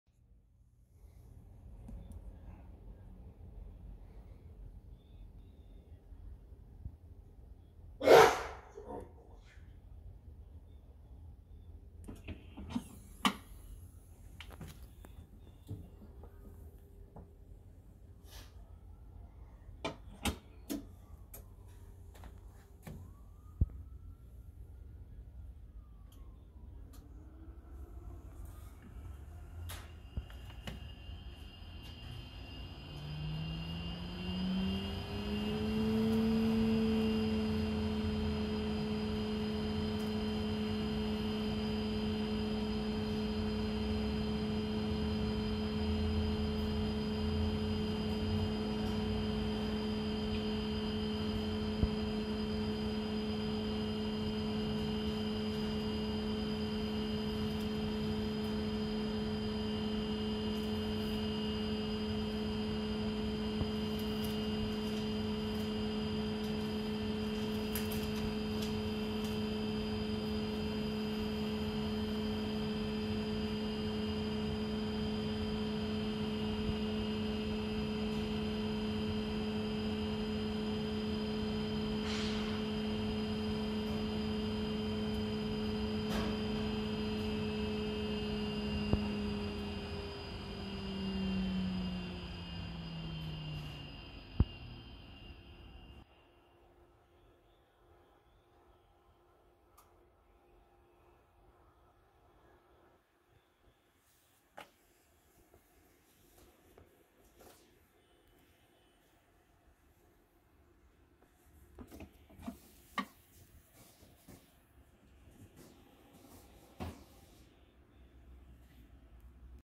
Thermo Scientific Sorvall ST16 Benchtop Centrifuge
This is a video to show that the centrifuge is in good physical and working condition.